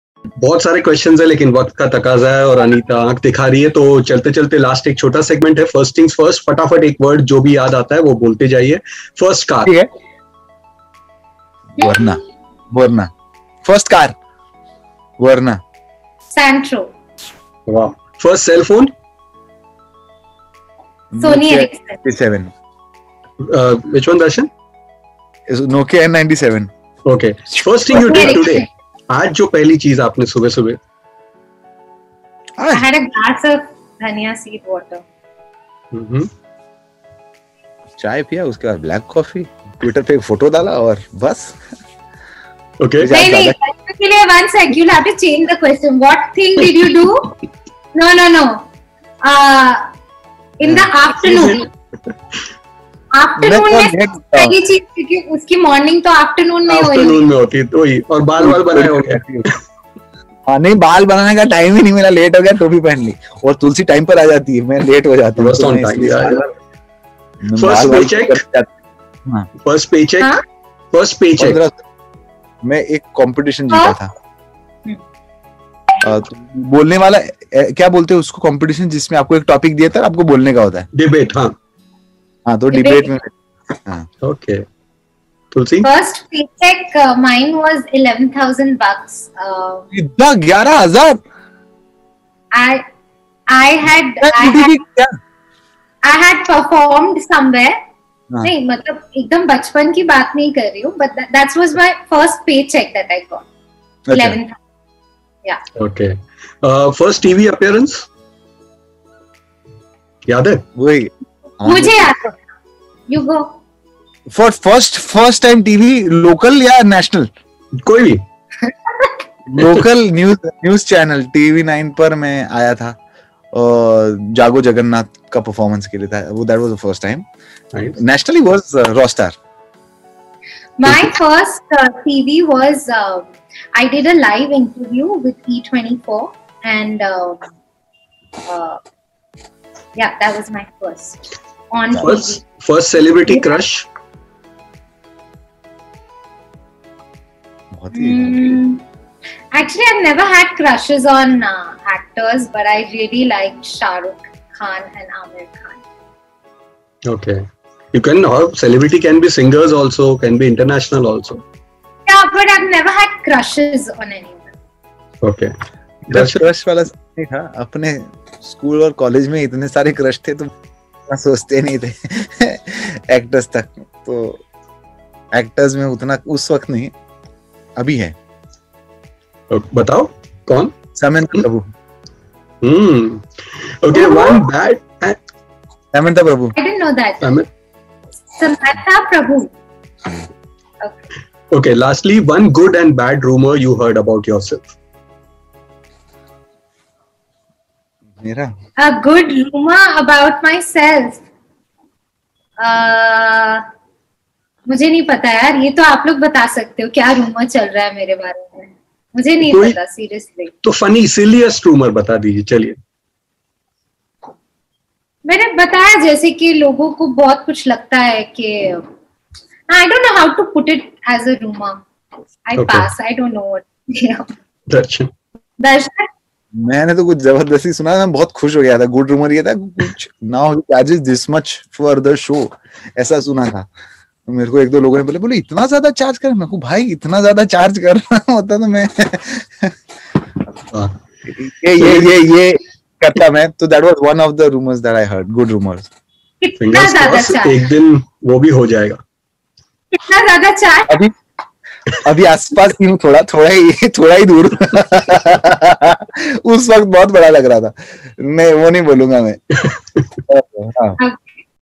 Rapid Fire with Darshan Raval & Tulsi Kumar|First Crush? First Love?|All Revealed!!!
Presenting here the "Rapid Fire" video of Darshan Raval & Tulsi Kumar

All Revealed!!!

Original Credits :-

The video is taken from Bollywood Spy. The video belongs to Bollywood Spy.